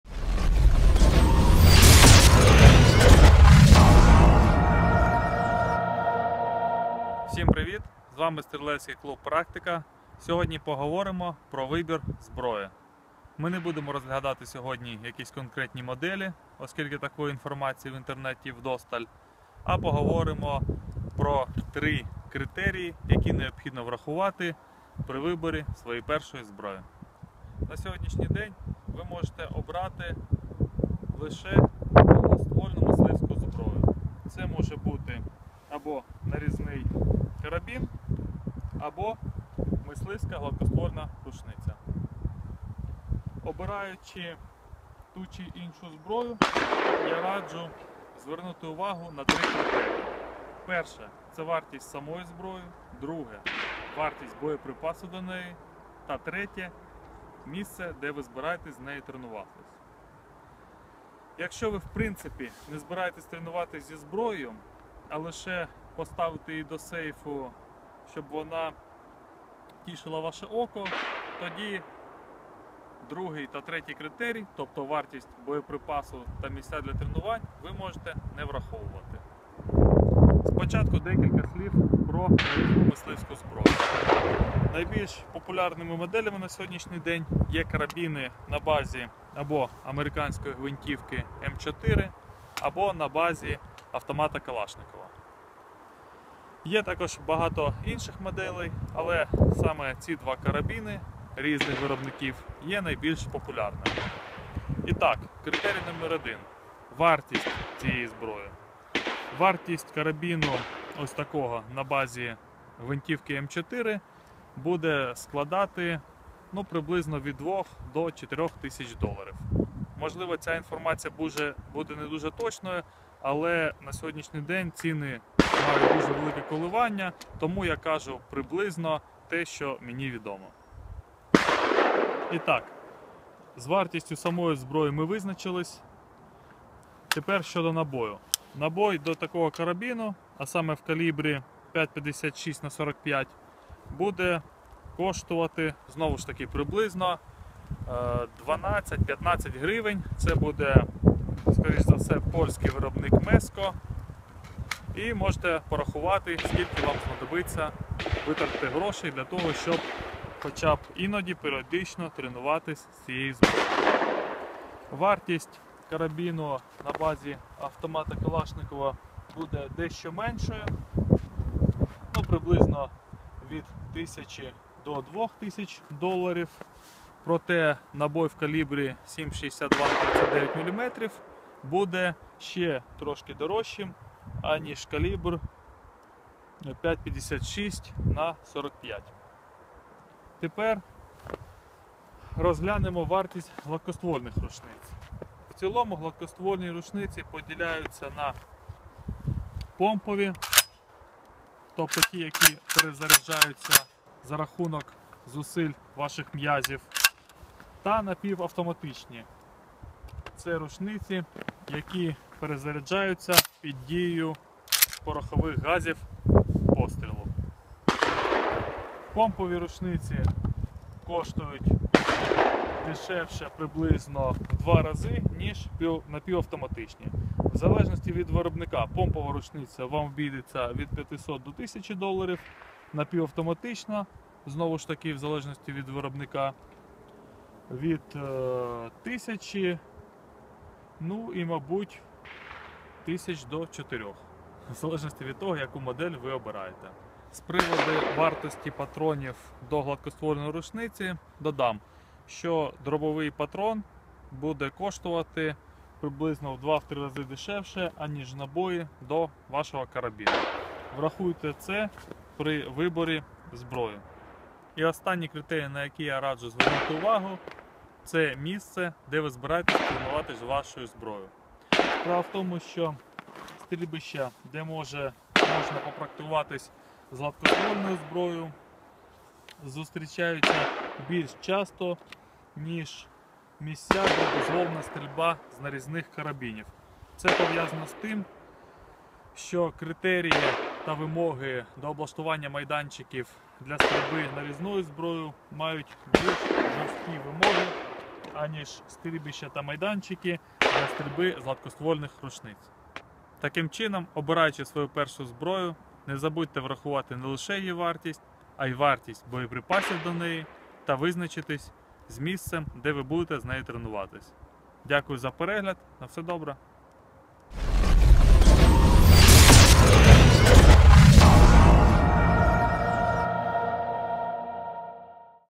Вибір першої зброї - що врахувати ?
При виборі Вашої першої зброї ми радимо врахувати не лише її вартість, а і вартість набоїв та визначитись з місцем для тренувань.